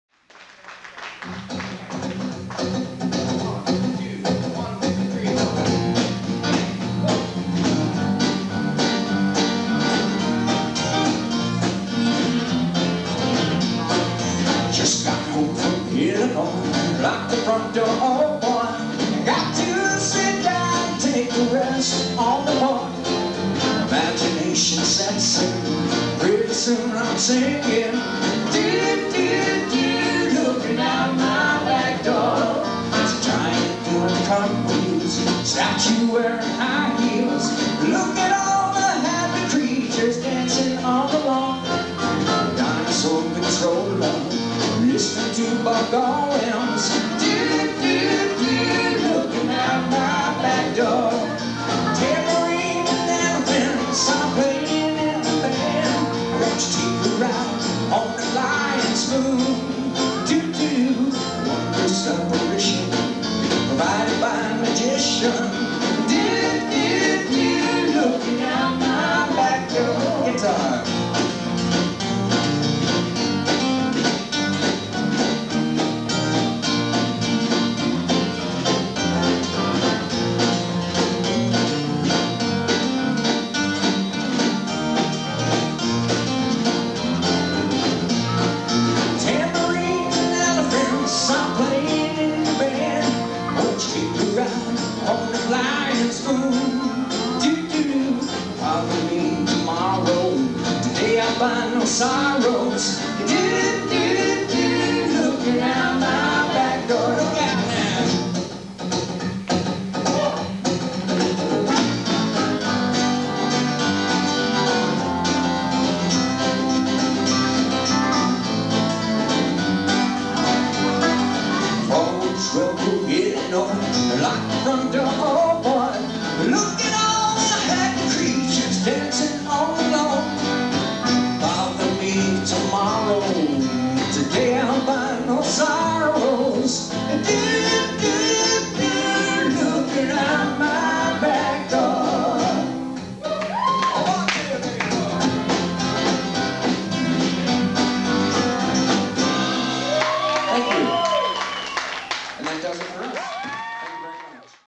Robbie Limon Band
CCR Cover - Fallen soldier's benefit, summer 2003